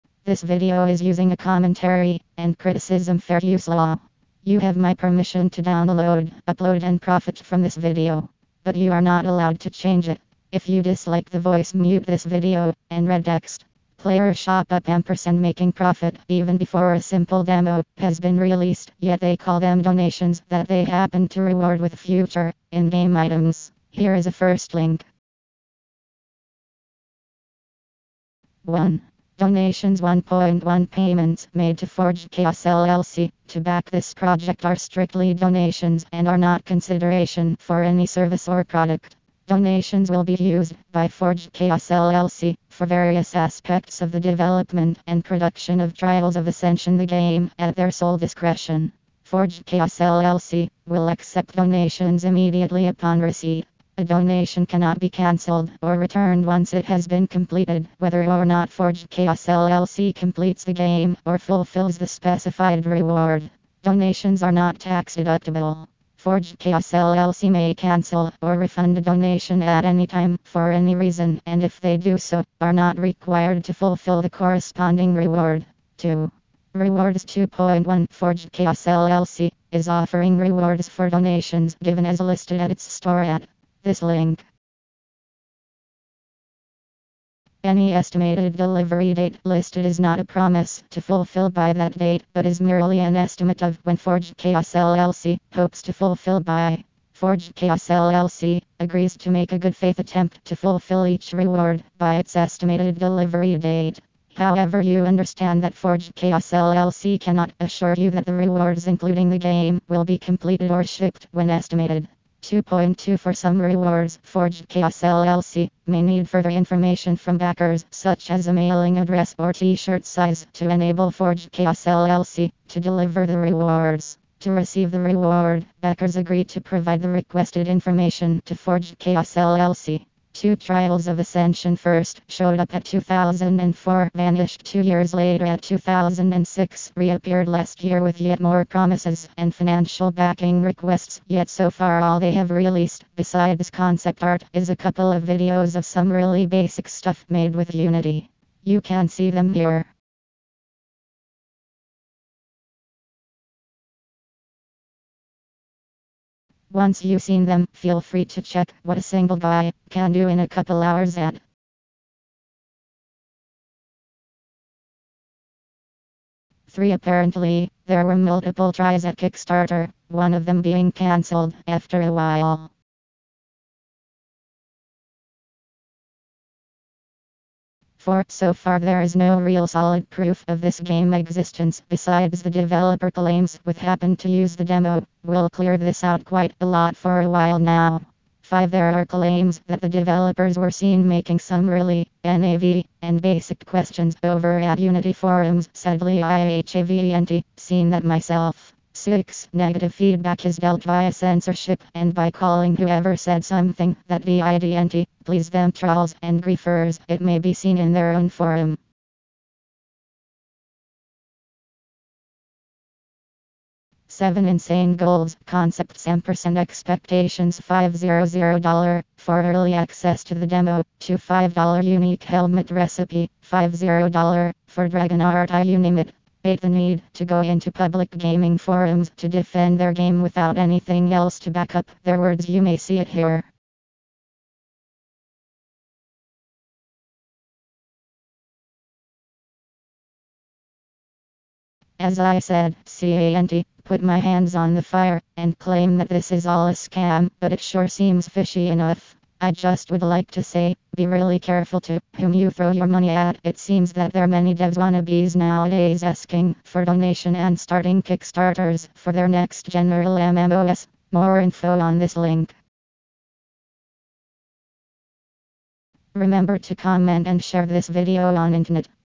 The most unbelievable indie fundraising scam to date. Trials of Ascension.
This video is exposing truth abauth the TOA. The sound is there just to make it more alive (you can mute) and its made like slideshow.

And no ... i will not take down this video!

You can reupload it on any site you want if the scamers manage to take down this video. (yes it will come back)

Watchout: Those people will remove any comment on their site or sites like Reddit. They have someone in party to remove any negative feedback. (instead on working on a actual game)
If you want you can read all the pages. but your head is gona hurt from stupidity.

I would love to see what the youtube start think of that.